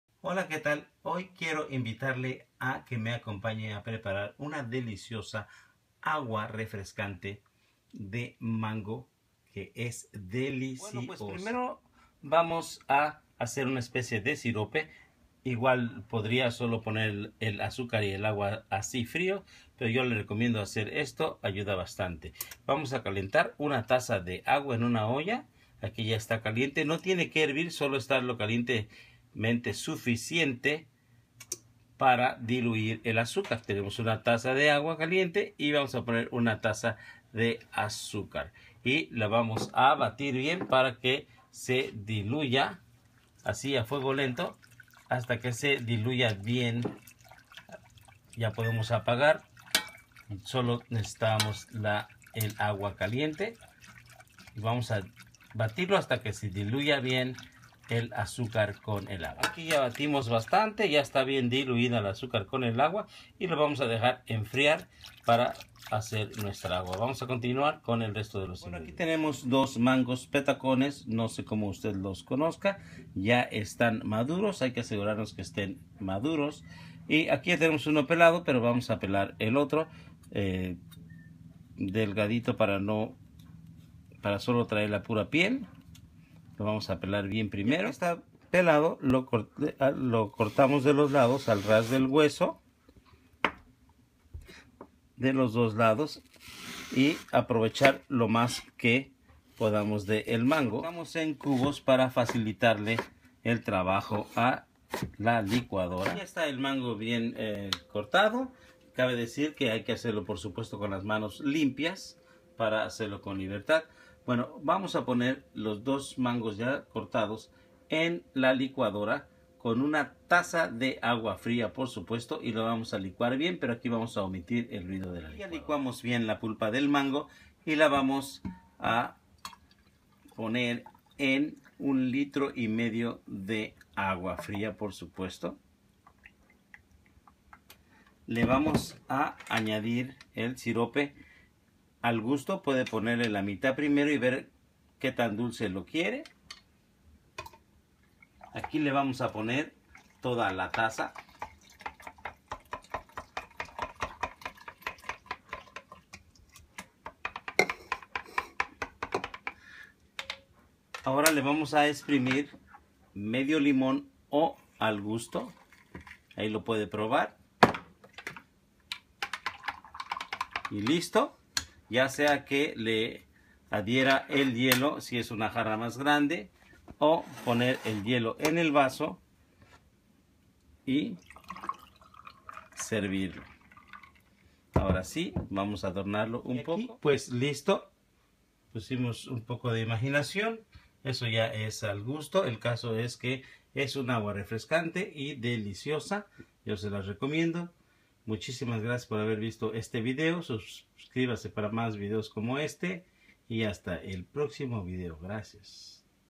AGUA FRESCA DE MANGO, RECETA DELICIOSA!
Como Hacer AGUA DE MANGO , Refrescante, Facil Y Deliciosa

Ingredientes;

2 litros de agua y 2 tassas extra
2 mangos petacones maduros
1 tasa de azucar
Jugo de medio limon o al gusto